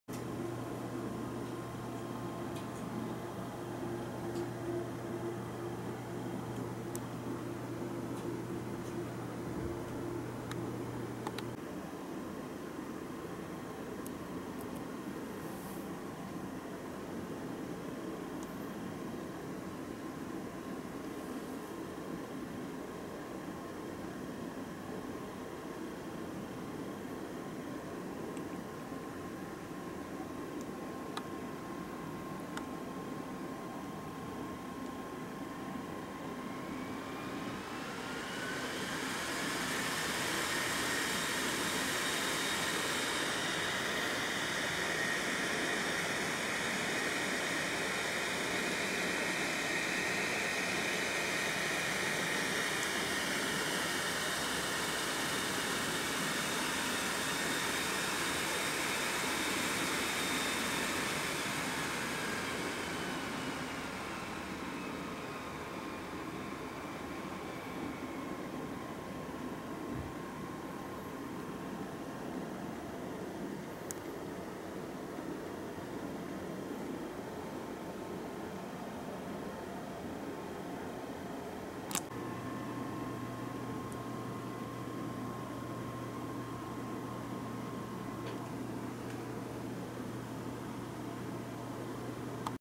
Modified BLDC Computer Blower
A Microchip MTD6501C 3 phase motor driver runs a modified computer blower.

The blower uses a hard drive motor plucked out of an old Samsung hard drive.  It is much quieter, more powerful, and more efficient than the stock 2 phase blower motor driver.

The 3 phase motor driver can take ~3-14V and is controlled via PWM from an ATTINY84 chip (programmed with Arduino) running on 5V.

 A LiFePo4 battery connected to a Pololu boost converter provides the 5V to run the circuit.